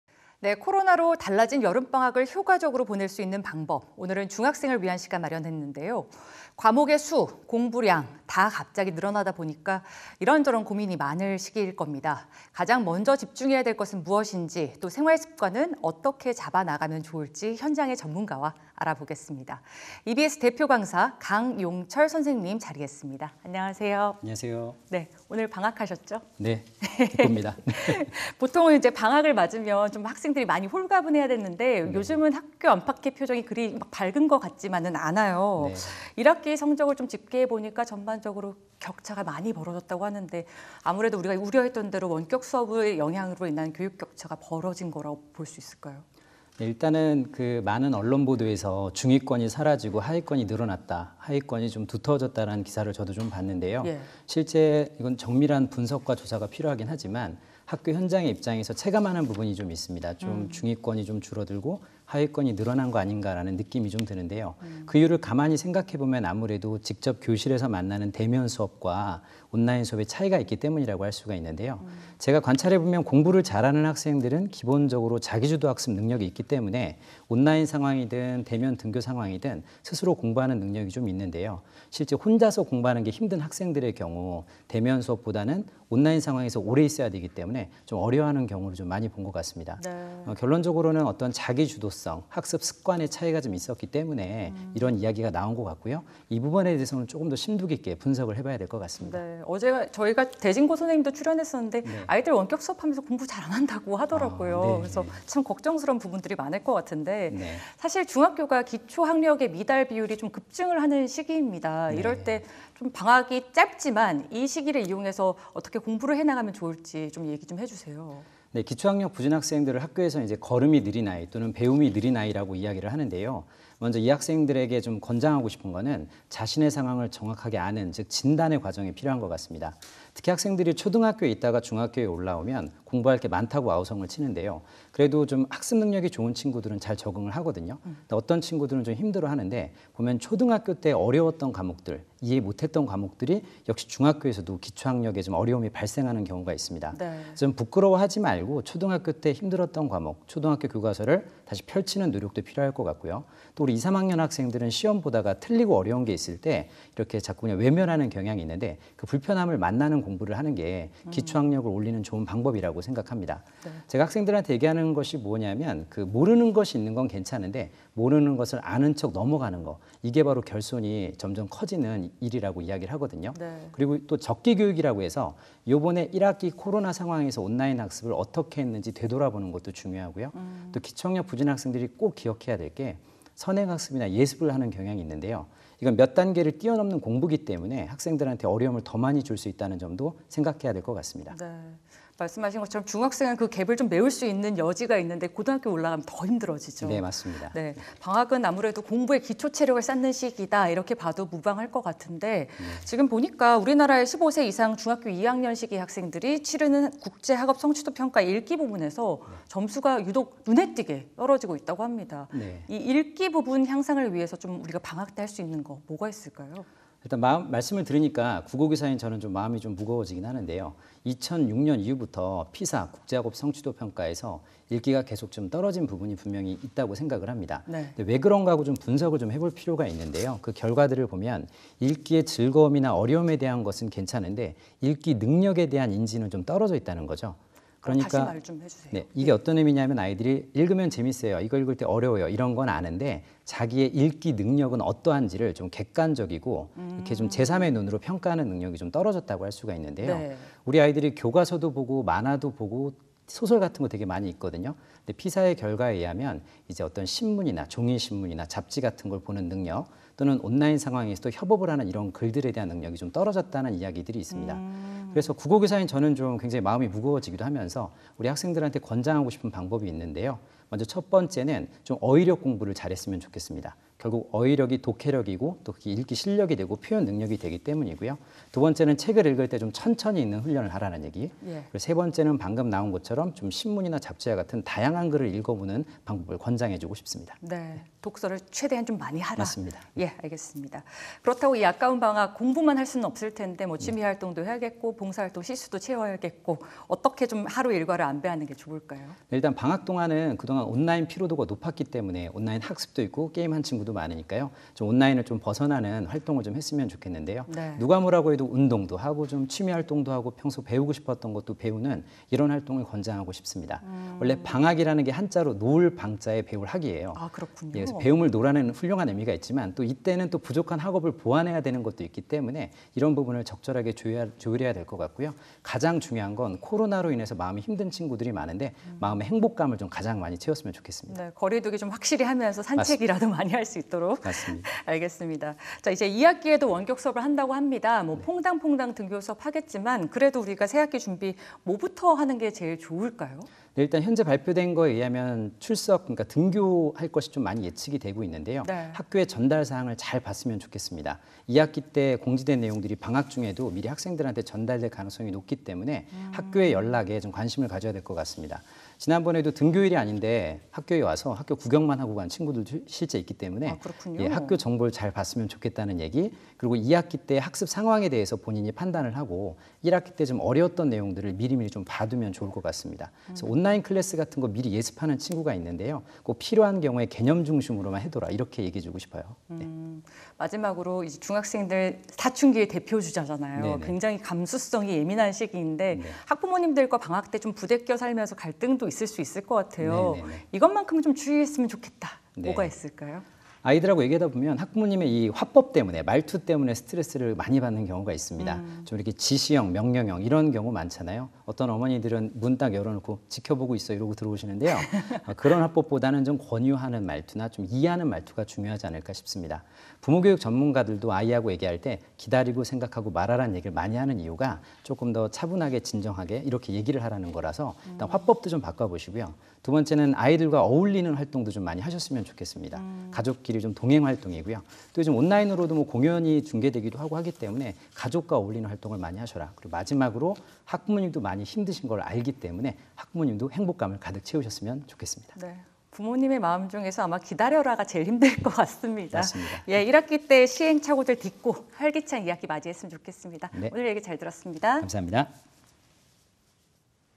2020.08.06 짧아진 방학 ‥ 중학생 공부·생활 어떻게?
유나영 아나운서

코로나로 달라진 여름방학을 효과적으로 보내는 법, 오늘은 중학생을 위한 시간입니다. 과목 숫자, 공부량, 다 갑자기 늘어나다 보니, 이런저런 고민이 많은 시기일 텐데요. 가장 먼저 집중해야 할 것은 무엇인지, 생활습관은 어떻게 잡아야 할지, 현장 전문가와 함께 자세히 알아봅니다. EBS 대표강사, 강용철 선생님 자리했습니다. 안녕하세요.

강용철 EBS 대표강사

안녕하세요.

유나영 아나운서

오늘 방학하셨죠. 방학을 맞았는데, 학교 안팎의 표정이 홀가분하지만은 않은 것 같습니다. 1학기 성적 집계해보니까, 전반적으로 격차가 벌어졌다고들 해요. 중위권에서 하위권으로 이동하는 학생들이 많았다고 하는데, 아무래도 원격수업의 영향이 컸다고 봐야겠습니까.

일단은 많은 언론 보도에서 중위권이 사라지고 하위권이 늘어났다, 하위권이 좀 두터워졌다는 기사를 저도 좀 봤는데요. 실제 정밀한 분석과 조사가 필요하긴 하겠지만, 학교 현장의 입장에서 체감하는 부분이 좀 있습니다. 중위권이 좀 줄어들고 하위권이 늘어난 것 아닌가라는 느낌이 좀 드는데요. 그 이유를 가만히 생각해보면 아무래도 직접 교실에서 만나는 대면 수업과 온라인 수업에 차이가 있기 때문이라고 할 수가 있는데요. 제가 관찰해보면 공부를 잘하는 학생들은 기본적으로 자기주도학습 능력이 있기 때문에 온라인 상황이든 대면 등교 상황이든 스스로 공부하는 능력이 있는데요. 실제 혼자서 공부하는 게 힘든 학생들의 경우, 대면 수업보다는 온라인 상황에서 오래 있어야 되기 때문에 좀 어려워하는 경우를 많이 본 것 같습니다. 결론적으로는 어떤 자기주도성, 학습 습관의 차이가 좀 있었기 때문에 이런 이야기가 나온 것 같고요. 이 부분에 대해서는 좀 더 심도 깊게 분석을 해봐야 될 것 같습니다.

유나영 아나운서

사실 중학교가 기초학력 미달 비율이 급증하는 시기입니다. 여러 가지로 어려움이 많은 시기예요. 특히, 공부가 힘들었던 학생들이라면 방학을 기회로 삼아야 할 텐데, 무엇부터 시작하면 좋을까요.

네, 기초학력 부진 학생들을 학교에서 걸음이 느린 아이, 또는 배움이 느린 아이라고 이야기하는데요. 먼저 이 학생들에게 권장하고 싶은 것은 자신의 상황을 정확하게 아는 진단의 과정이 필요한 것 같습니다. 특히 학생들이 초등학교에 있다가 중학교에 올라오면 공부할 게 많다고 아우성을 치는데요. 그래도 좀 학습 능력이 좋은 친구들은 잘 적응하거든요. 어떤 친구들은 좀 힘들어하는데, 보면 초등학교 때 어려웠던 과목들, 이해 못 했던 과목들이 역시 중학교에서도 기초학력에 어려움이 발생하는 경우가 있습니다. 부끄러워하지 말고 초등학교 때 힘들었던 과목, 초등학교 교과서를 다시 펼치는 노력이 필요할 것 같고요. 또 2, 3학년 학생들은 시험 보다가 틀리고 어려운 게 있을 때 자꾸 외면하는 경향이 있는데 불편함을 만나는 공부를 하는 게, 기초학력을 올리는 좋은 방법이라고 생각합니다. 제가 학생들한테 얘기하는 것이 뭐냐면, 모르는 것이 있는 건 괜찮은데, 모르는 것을 아는 척 넘어가는 것, 이게 바로 결손이 점점 커지는 일이라고 이야기하거든요. 그리고 또 적기 교육이라고 해서 이번에 1학기 코로나 상황에서 온라인 학습을 어떻게 했는지 되돌아보는 것도 중요하고요. 또 기초학력 부진 학생들이 또 기억해야 될 게, 선행학습이나 예습을 하는 경향이 있는데요. 몇 단계를 뛰어넘는 공부기 때문에, 학생들한테 어려움을 더 많이 줄 수 있다는 점도 생각해야 될 것 같습니다.

유나영 아나운서

방학은 공부의 기초체력 쌓기도 좋은 시기죠. 예를 들면, 읽기 능력입니다. 우리나라 15세 이상, 딱 중학교 2학년 시기인데요. 국제학업성취도평가 읽기 부문에서 유독 점수가 계속 떨어지고 있다고 해요. 학기 중엔 시험 준비하느라 바쁘고, 방학 때 보완할 방법이 있을까요?

말씀을 들으니까 국어교사인 저는 마음이 좀 무거워지긴 하는데요. 2006년 이후부터 피사, 국제학업성취도평가에서 읽기가 계속 떨어진 부분이 분명히 있다고 생각을 합니다. 왜 그런가 하고 분석을 해볼 필요가 있는데요. 그 결과들을 보면 읽기의 즐거움이나 어려움에 대한 것은 괜찮은데 읽기 능력에 대한 인지는 좀 떨어져 있다는 거죠. 이게 어떤 의미냐면 아이들이 읽으면 재미있어요, 이거 읽을 때 어려워요 이런 건 아는데, 자기의 읽기 능력은 어떠한지를 객관적이고 제삼의 눈으로 평가하는 능력이 좀 떨어졌다고 할 수 있는데요. 우리 아이들이 교과서도 보고 만화도 보고, 소설 같은 것도 되게 많이 읽거든요. 피사의 결과에 의하면 어떤 신문이나, 종이 신문이나 잡지 같은 것을 보는 능력, 또는 온라인 상황에서도 협업을 하는 이런 글들에 대한 능력이 좀 떨어졌다는 이야기들이 있습니다. 그래서 국어교사인 저는 좀 굉장히 마음이 무거워지기도 하면서, 우리 학생들한테 권장하고 싶은 방법이 있는데요. 먼저 첫 번째는 어휘력 공부를 잘했으면 좋겠습니다. 결국 어휘력이 독해력이고 읽기 실력이 되고 표현 능력이 되기 때문이고요. 두 번째는 책을 읽을 때 좀 천천히 읽는 훈련을 하라는 얘기, 세 번째는 방금 나온 것처럼 신문이나 잡지와 같은 다양한 글을 읽어 보는 방법을 권장해주고 싶습니다.

유나영 아나운서

그렇다고 공부만 할 수는 없지 않습니까. 취미활동도 해야 하고, 코로나 때문에 제한적이긴 하지만 봉사활동이라든지, 할 일이 많을 것 같아요. 하루 일과는 어떻게 안배를 하면 좋을까요?

일단 방학 동안은, 그동안 온라인 피로도가 높았기 때문에, 온라인 학습도 있고 또 게임하는 친구도 많으니까요. 온라인을 좀 벗어나는 활동을 했으면 좋겠는데요. 누가 뭐라고 해도 운동도 하고 취미 활동도 하고 평소 배우고 싶었던 것도 배우는, 이런 활동을 권장하고 싶습니다. 원래 방학이라는 게 한자로 놓을 방자에 배울 학이에요. 배움을 놓으라는 아주 훌륭한 의미가 있지만 또 이때는 부족한 학업을 보완해야 되는 것도 있기 때문에 이런 부분을 적절하게 조율해야 될 것 같고요. 가장 중요한 건 코로나로 인해서 마음이 힘든 친구들이 많은데, 마음의 행복감을 가장 많이 챙겼으면 좋겠습니다.

유나영 아나운서

2학기에도 원격수업을 한다고 합니다. 학교 나가는 날은 조금 늘어나겠지만, 어쨌든 또 퐁당퐁당 등교예요. 새 학기 준비는 어떻게 하는 것이 좋을까요?

일단 현재 발표된 것에 의하면 출석, 등교할 것이 많이 예측이 되고 있는데요. 학교의 전달사항을 잘 봤으면 좋겠습니다. 2학기 때 공지된 내용들이 방학 중에도 미리 학생들한테 전달될 가능성이 높기 때문에 학교의 연락에 관심을 가져야 될 것 같습니다. 지난번에도 등교일이 아닌데, 학교에 와서 학교 구경만 하고 간 친구도 실제 있기 때문에 학교 정보를 잘 봤으면 좋겠다는 얘기, 그리고 2학기 때 학습 상황에 대해서 본인이 판단을 하고, 1학기 때 어려웠던 내용들을 미리미리 봐두면 좋을 것 같습니다. 온라인 클래스 같은 것 미리 예습하는 친구가 있는데요. 필요한 경우에 개념 중심으로만 해둬라, 이렇게 얘기해주고 싶어요.

유나영 아나운서

중학생들이 또 정서적으로 예민하죠. 방학은 아무래도 서로 부대끼는 시간이 많으니까요. 이런저런 갈등이 많아지기도 합니다. 마지막으로 사춘기 대표 주자, 우리 중학생 아이들과 좋은 관계를 유지하려면, 학부모들이 배려해야 할 점은 무엇인지, 조언해주실 수 있을까요?

아이들하고 얘기하다 보면 학부모님이 이 화법 때문에, 말투 때문에 스트레스를 많이 받는 경우가 있습니다. 지시형, 명령형, 이런 경우 많잖아요. 어떤 어머니들은 문 딱 열어놓고 지켜보고 있어, 이러고 들어오시는데요. 그런 화법보다는 좀 권유하는 말투나 이해하는 말투가 중요하지 않을까 싶습니다. 부모 교육 전문가들도 아이하고 얘기할 때 기다리고 생각하고 말하라는 얘기를 많이 하는 이유가, 조금 더 차분하게 진정하게 얘기를 하라는 거라서, 화법도 좀 바꿔보시고요. 두 번째는 아이들과 어울리는 활동을 좀 많이 하셨으면 좋겠습니다. 가족끼리 동행 활동이고요. 또 온라인으로도 공연이 준비되고 하기 때문에 가족과 어울리는 활동을 많이 하셔라, 마지막으로 학부모님도 많이 힘드신 걸 알기 때문에, 학부모님도 행복감을 가득 채우셨으면 좋겠습니다.

유나영 아나운서

네, 부모님의 마음 중에서 아마 기다려라가 제일 힘들 것 같습니다. 1학기 때 시행착오들 딛고, 활기찬 2학기 맞이했으면 좋겠습니다. 오늘 얘기 잘 들었습니다.

감사합니다.